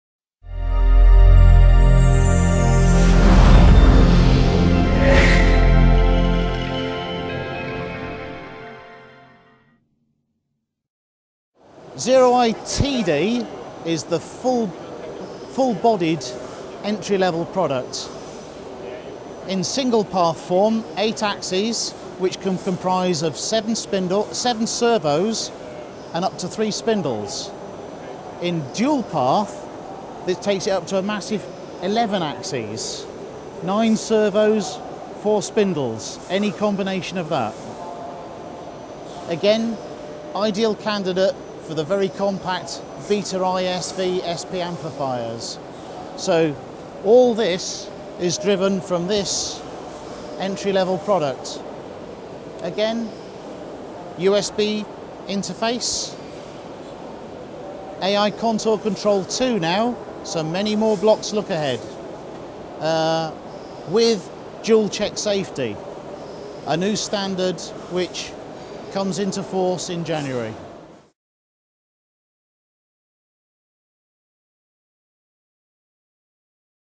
FANUC 0i-TD @ EMO Hannover 2011 [ENG]
Introduction of the Fanuc 0i-TD CNC controller by Ian Baird, Application Manager of FANUC FA Europe SA at EMO Hannover 2011.